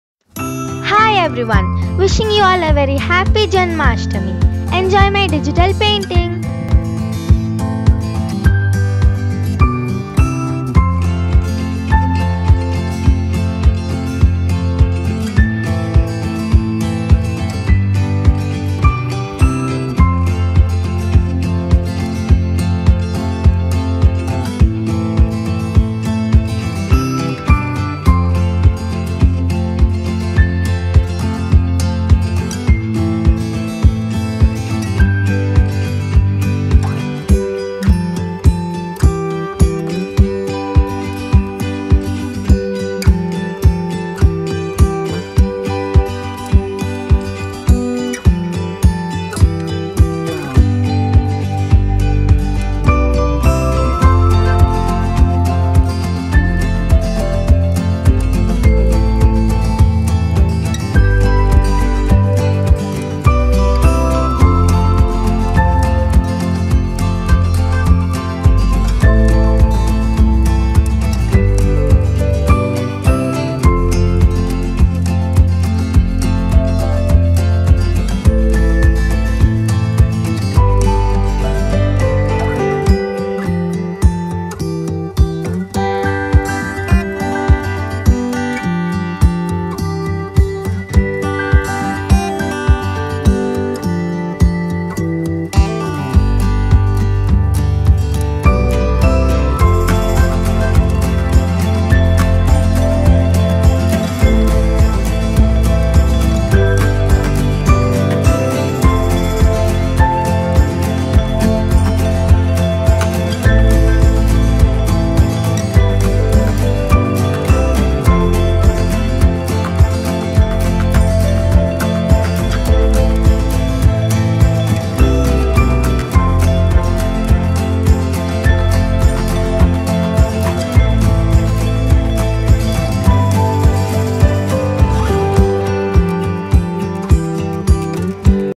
Krishna Janmashtami || Krishna Jayanthi || Paint 3D || Digital Painting || TANUJA'S CREATION
Wishing you all a very Happy Janmashtami. Enjoy this day by watching my Digital Painting Video